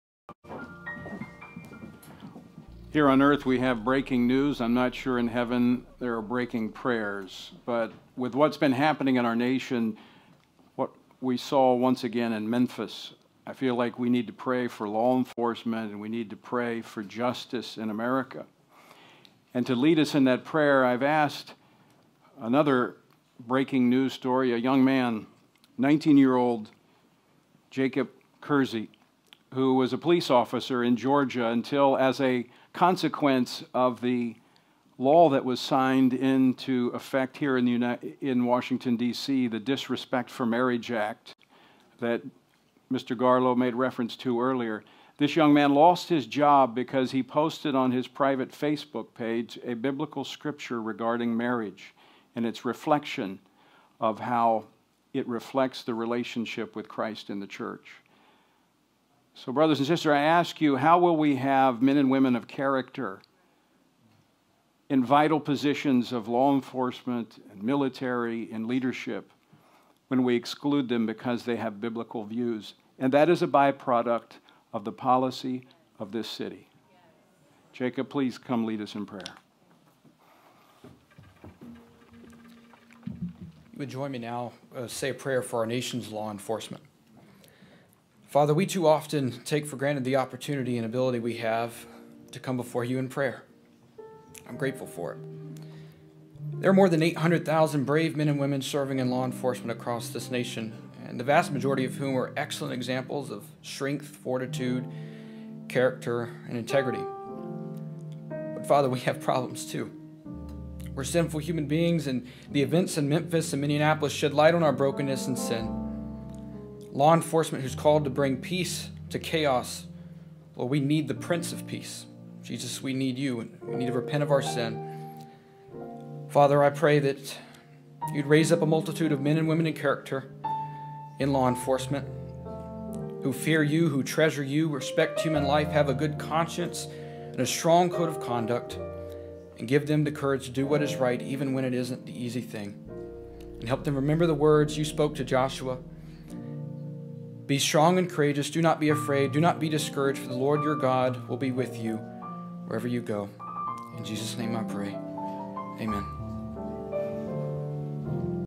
Former Officer Jacob Kersey Prays For Our Nation’s Law Enforcement
Tony Perkins introduces Jacob Kersey at the National Gathering For Prayer And Repentance which was held at the Museum of the Bible in Washington D.C.

Jacob then prays for the nation’s law enforcement in light of the recent events in Minneapolis and Memphis.